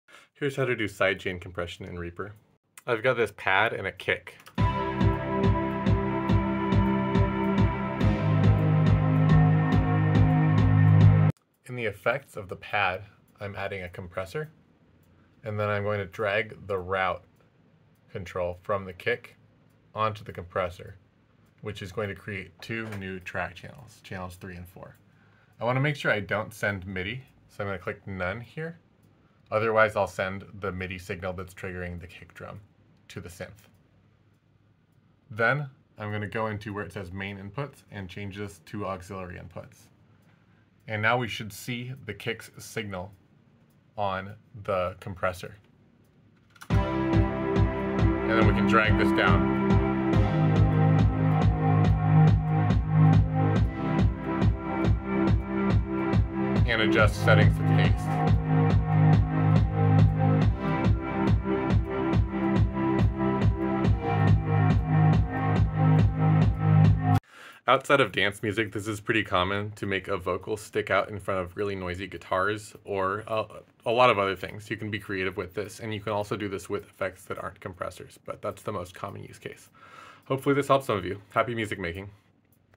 How to do Side Chain Compression in Reaper (and how to stop your kick from triggering the synth!)
A brief overview of the most common use case for using a compressor's side chain input, using Reaper as our DAW and the stock compressor plugin ReaComp as our compressor.

Note that you can also do this with normal routing; Reaper just makes it easier on us by letting us create new track channels when we drag the "Route" control directly to the plugin (as opposed to dragging it to the track we're targeting).

Happy music making!